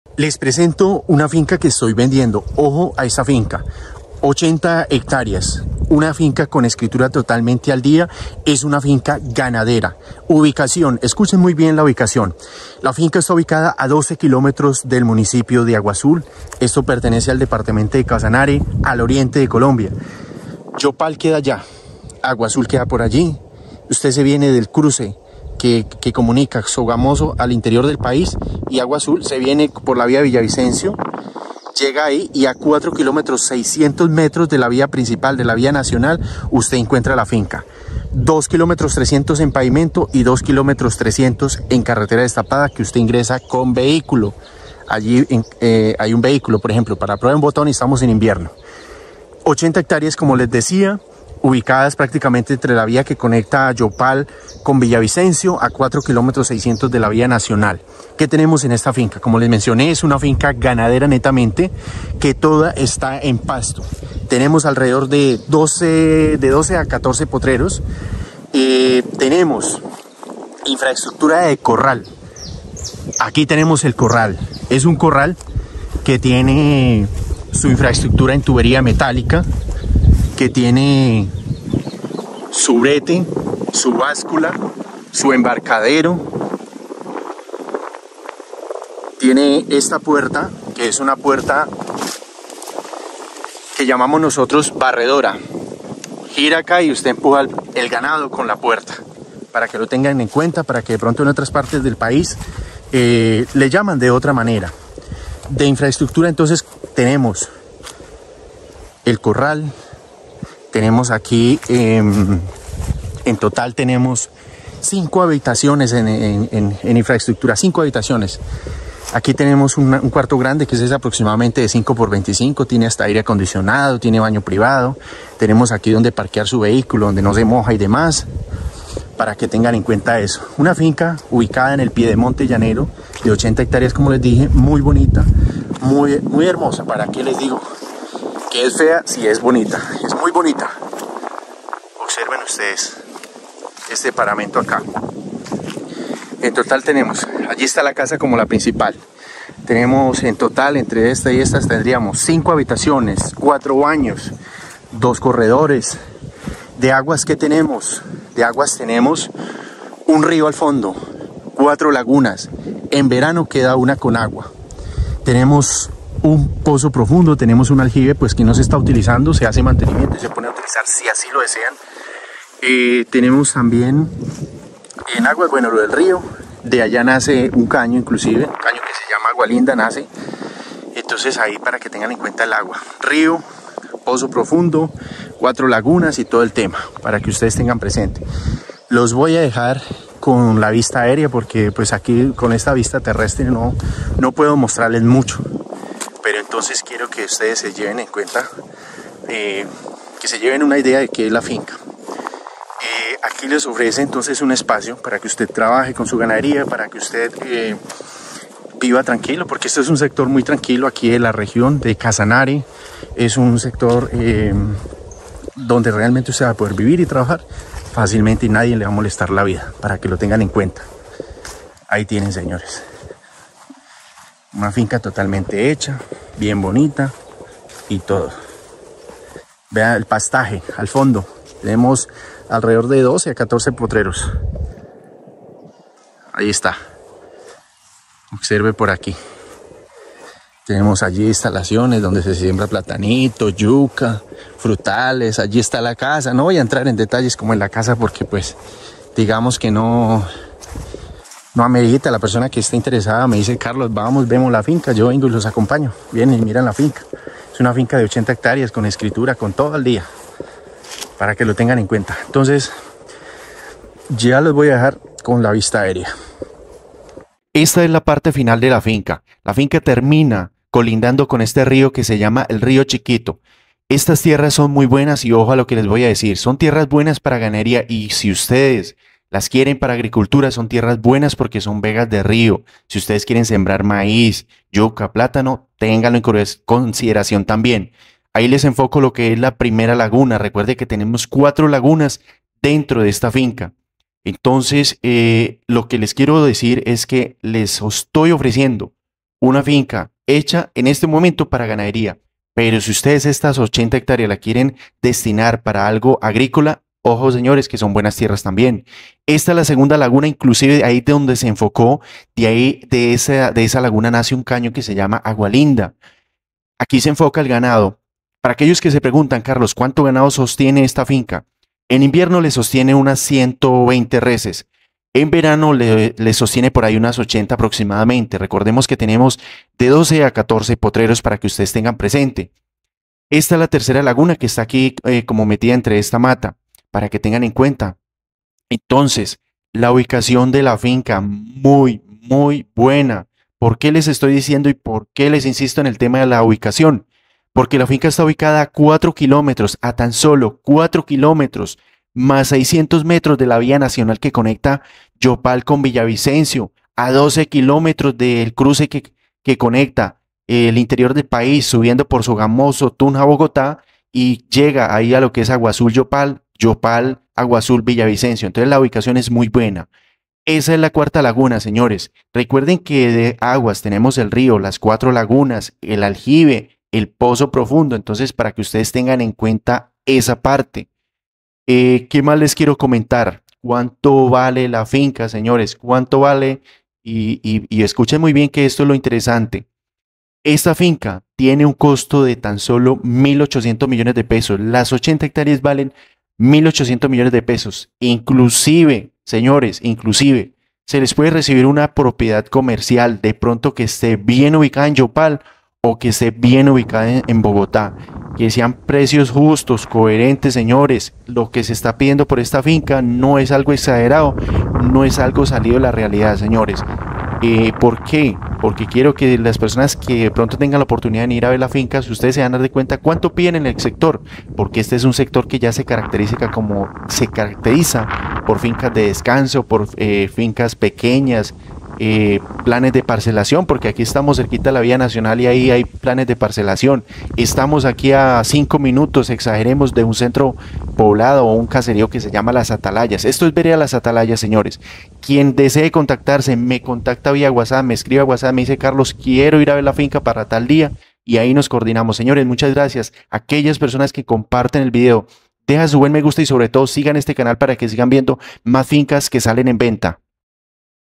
🔥PROPIEDAD VENDIDA🔥
🔥(SE VENDE)🔥FINCA DE: (80) HECTÁREAS👩‍🌾Con posibilidad de recibir: propiedad comercial de menor valor en Bogotá o Yopal🔥

UBICACIÓN:
La finca está ubicada a tan solo (12) kilómetros del área urbana del municipio de “Aguazul” (Dpto. de Casanare). Exactamente está a (4.6) kilómetros de la vía nacional que conecta “Yopal” con “Villavicencio”, vereda “Las Atalayas”.

Está cerca del caserío o centro poblado “Las Atalayas”. (2.3) kilómetros tiene en carretera destapada, ingresan vehículos pequeños sin problema alguno. La finca está a unos 35 minutos aproximadamente de la ciudad de “Yopal”.

COMPOSICIÓN Y DETALLES:
El área total es de: (80) hectáreas, la topografía es plana, clima cálido, son tierras frescas para la época del verano. Actualmente la finca está acondicionada para ganadería. Prácticamente toda está en pastos mejorados.

Se cuenta con: (12) potreros, donde se sostienen un promedio de 120 reses en invierno y unas 80 reses en época de verano.

En infraestructura se tiene: (2) casas que en total suman: (5) habitaciones, (4) baños, (2) cocinas, (2) corredores, (1) parqueadero cubierto para 2 vehículos. (1) caballeriza, (1) cuarto para herramientas, (1) corral en tubería metálica (con báscula, brete y puerta barredora).

La finca cuenta con: red de energía eléctrica, red de gas domiciliario, (1) pozo profundo (a 25 metros de profundidad aprox.), (1) aljibe y (4) lagunas. Dentro de la finca nace un caño y se cuenta con acceso a un río de nombre “Río Chiquito”.

LA FINCA VALE:
COP🇨🇴: $1.800 millones en total.
Con posibilidad de recibir: propiedad comercial de menor valor en Bogotá o Yopal.